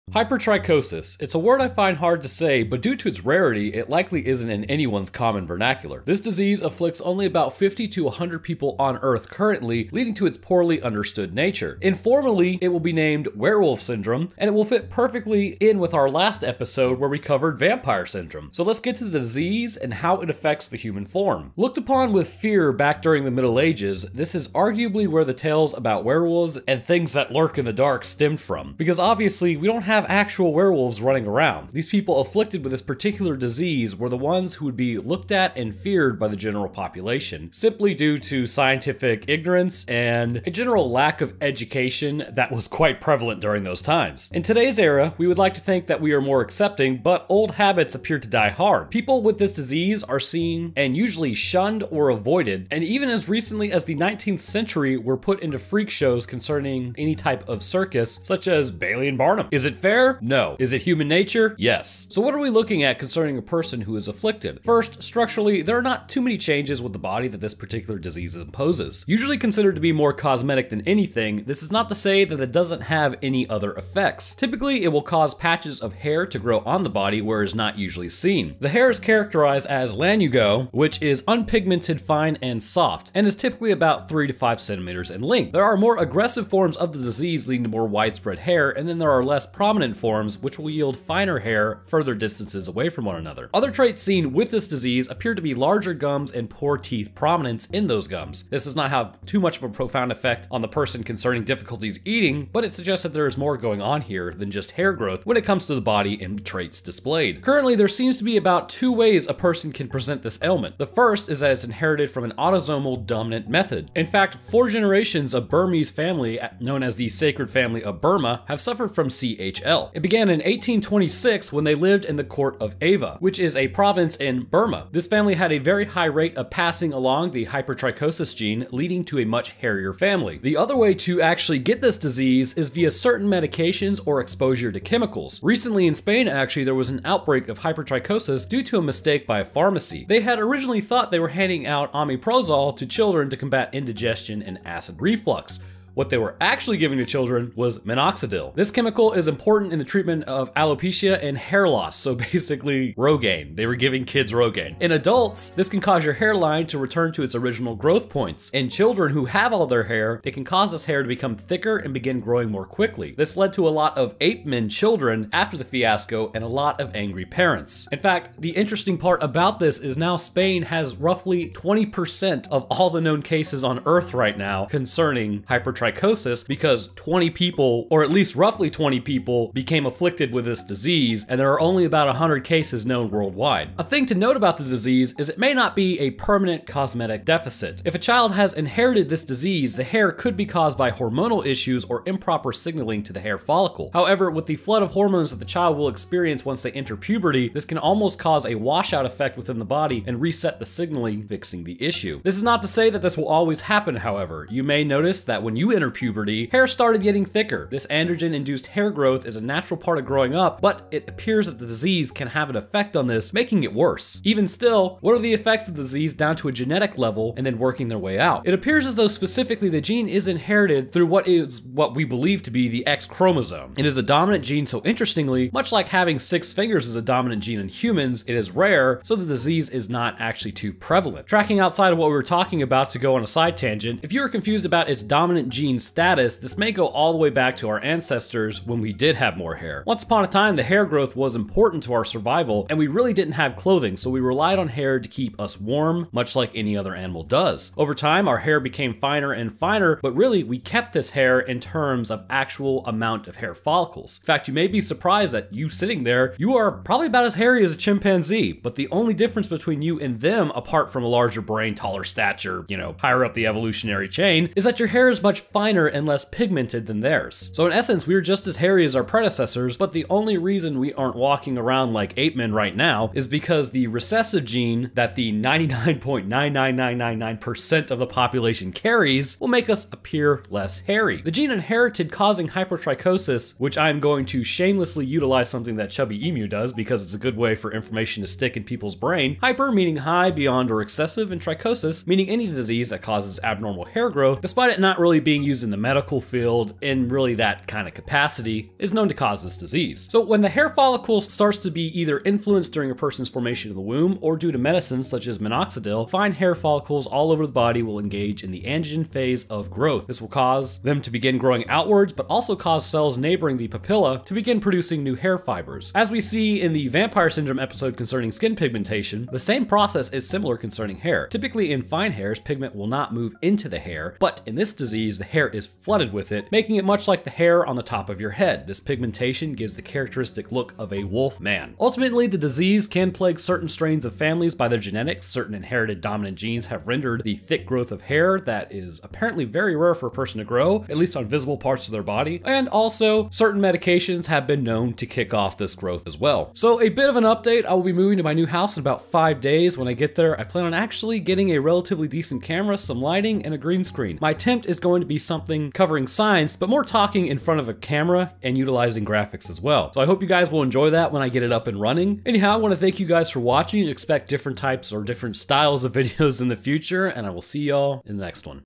Werewolf Syndrome Explored | The disease known as Hypertrichosis Explained
Hypertrichosis is a disease that is so exceedingly rare, only about 100 to 150 cases are known worldwide. In this episode we will discuss what are the effects and where it comes from!